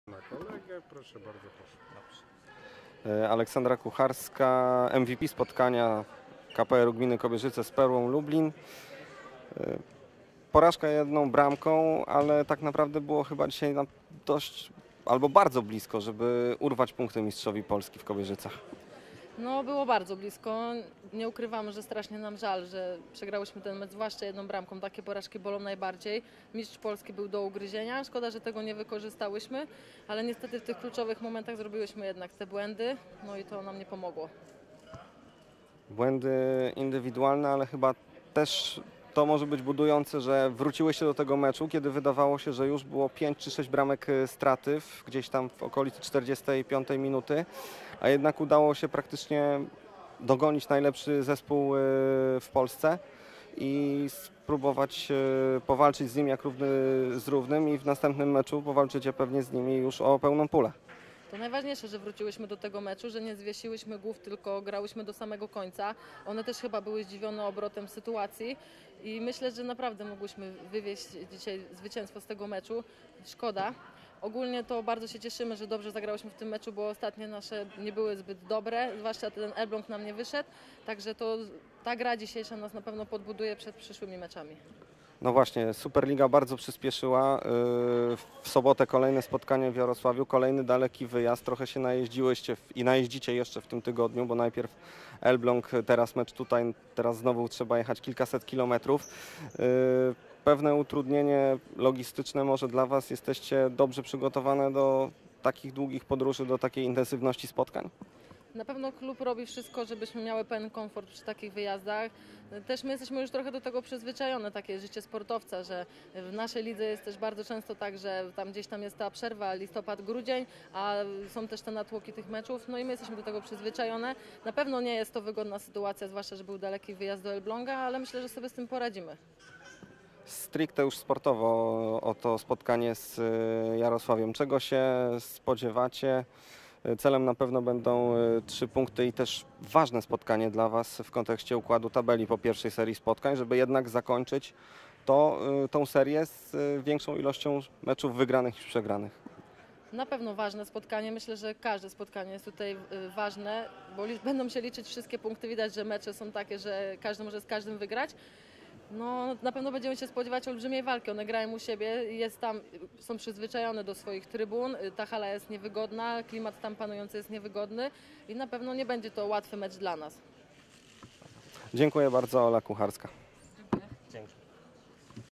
KPR Kobierzyce vs Perła Lublin Wywiad pomeczowy z MVP A Kucharską
6 seria Superligi w sezonie 2019/20. 23 października 2019. Mecz KPR Kobierzyce vs. MKS Perła Lublin. MVP spotkania. Aleksandra Kucharska.